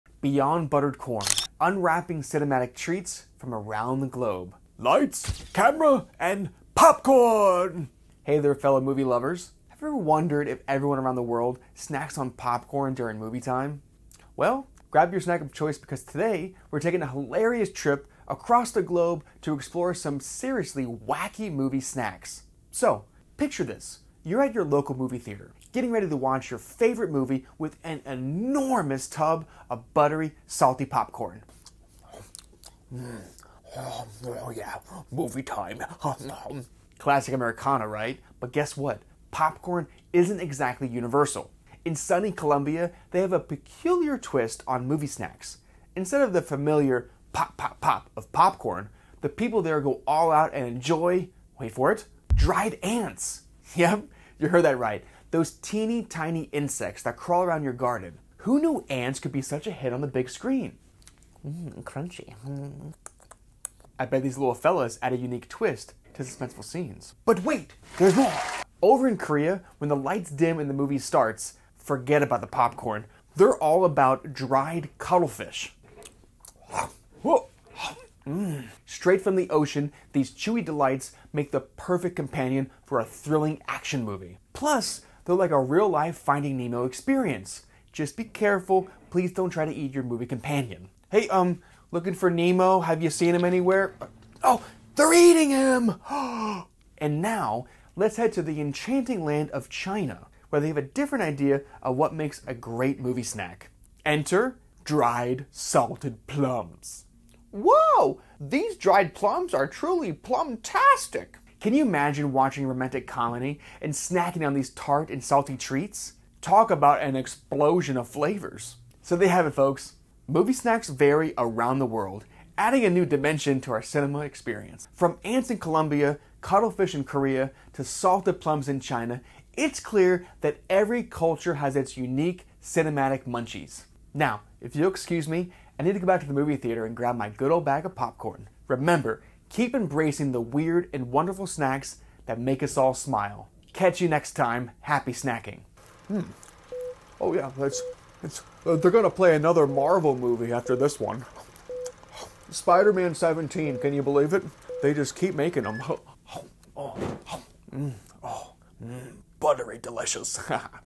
Beyond Buttered Corn: Unwrapping Cinematic Treats from Around the Globe | Dr Brett Berner
Join our cheerful host on a global journey to explore quirky movie snacks from different corners of the world. From dried ants in Colombia to cuttlefish in Korea and dried salted plums in China, discover the unique flavors that accompany cinema experiences worldwide. Embrace the weird and wonderful snacks that make movie-watching an international delight! 🍿🌍